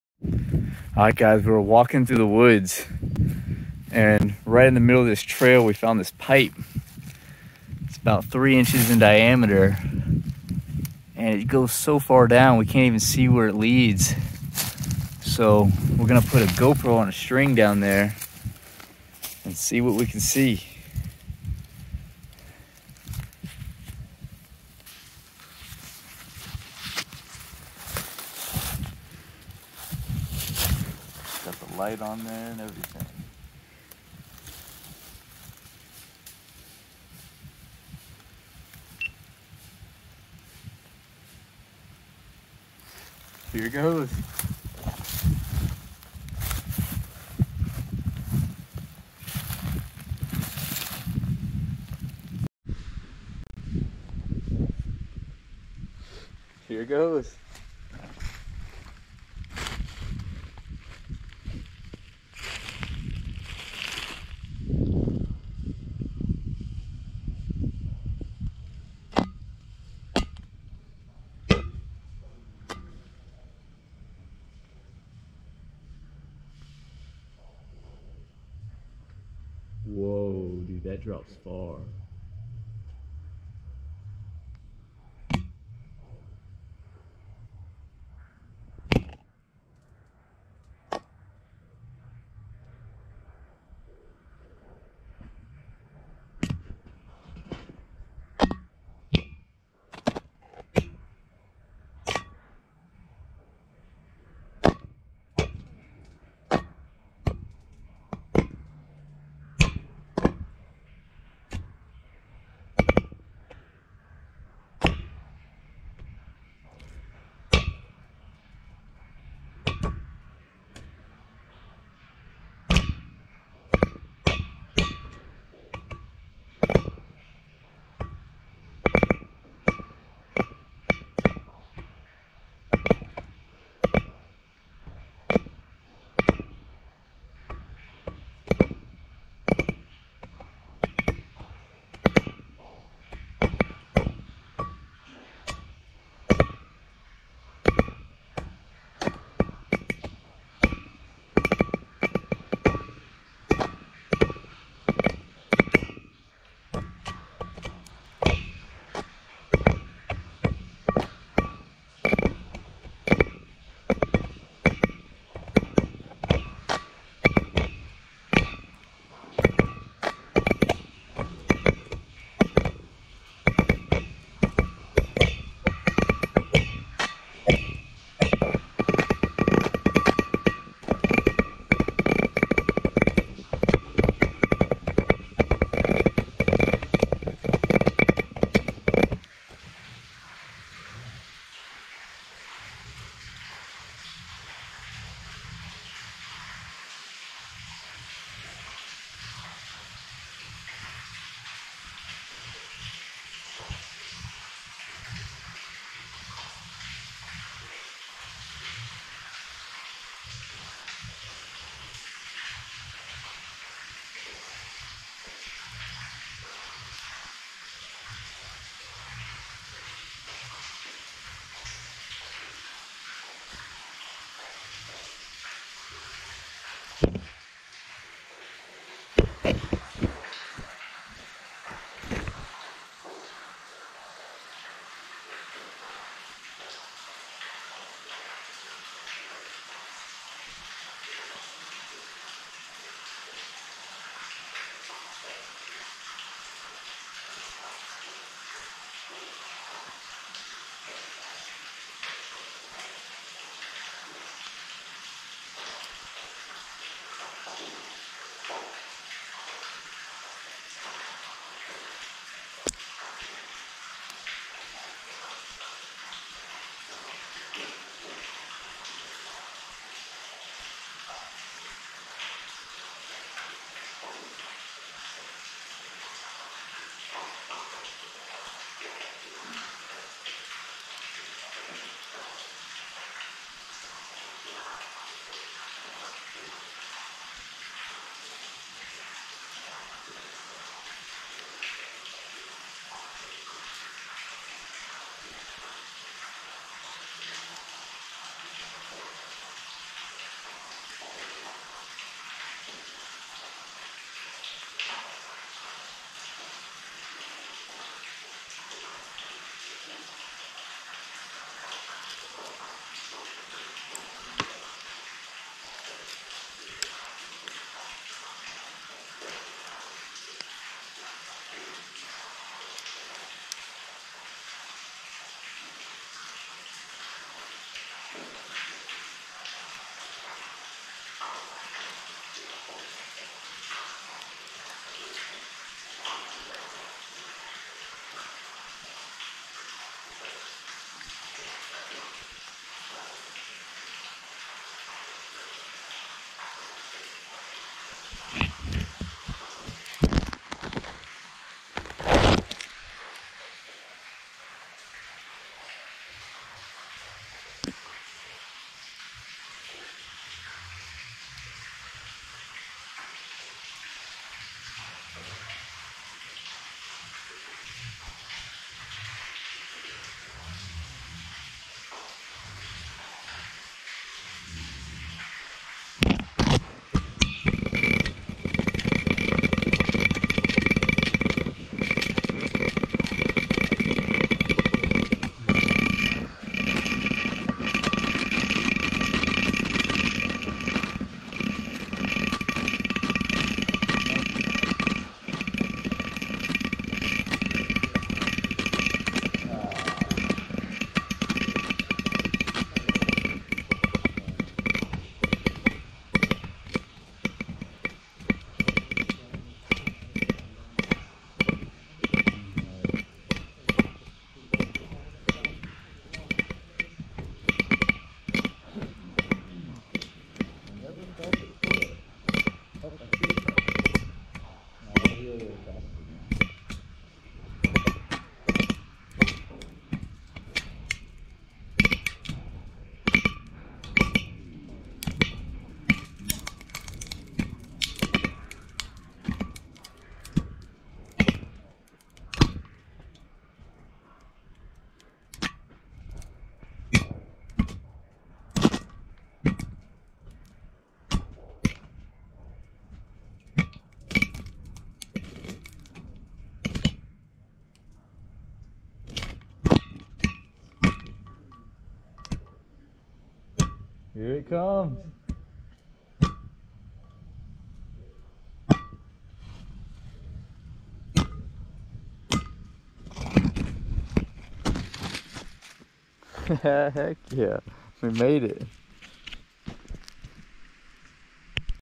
Dropping Go Pro Down A Deep Mystery Pipe
Our 125 foot/40m long rope just barely reached the bottom when it opened up.
we think it leads into an abandoned mine